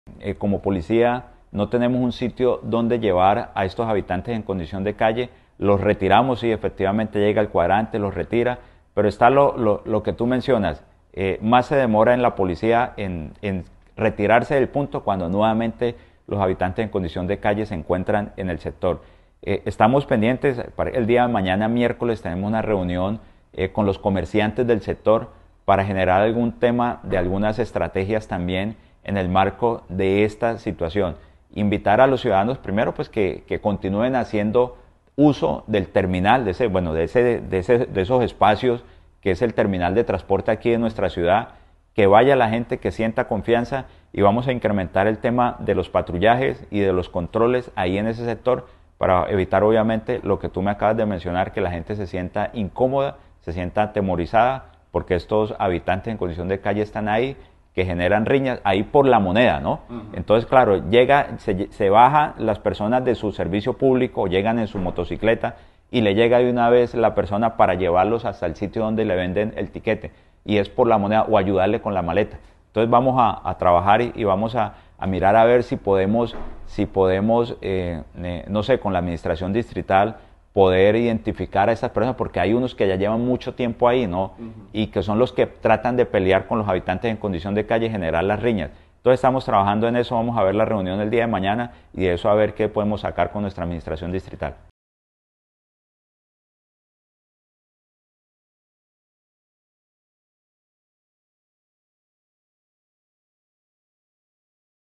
Reunión con comerciantes para establecer acciones que permitan intervenir problemática de seguridad
Las autoridades de Policía informaron que se realizará una reunión con comerciantes del sector de las empresas de transporte terrestre, para establecer acciones que permitan intervenir la problemática de inseguridad causada por los habitantes de calle. Veamos las declaraciones del coronel Luis Alejandro cubillos, comandante de la Policía del Magdalena Medio.